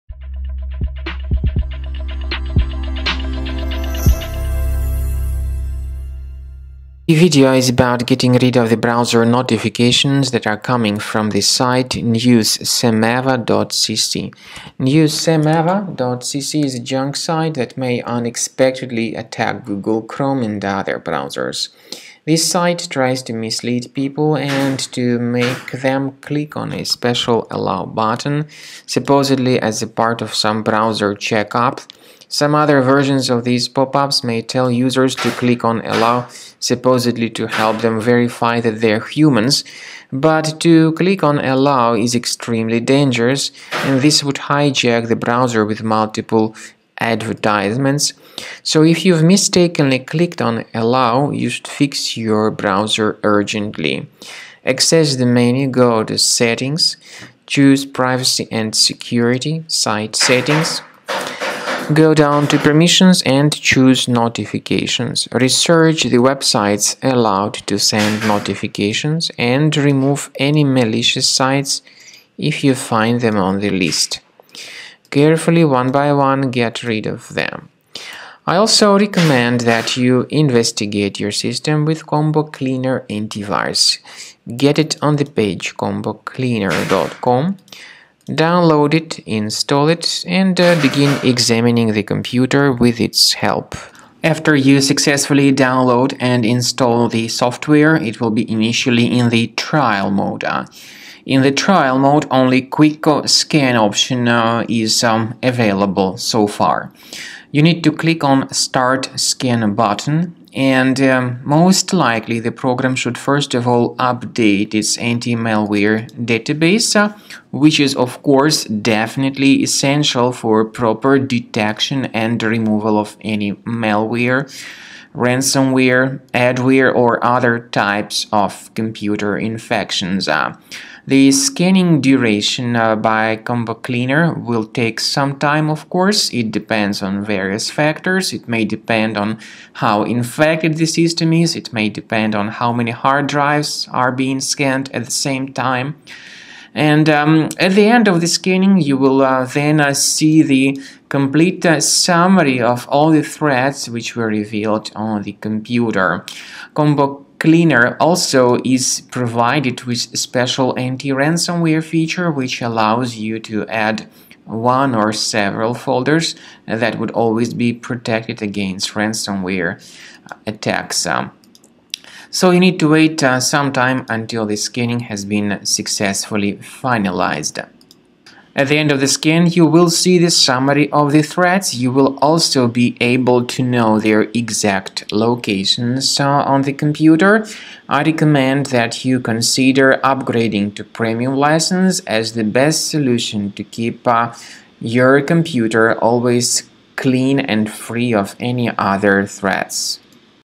News-samava.cc web push notifications - how to remove?
News-samava.cc is a dangerous site. It attacks Google Chrome and other browsers. Don't tolerate these fake alerts. Get rid of them with Combo Cleaner Antivirus via the official or, optionally, follow the manual suggestions provided in the video tutorial.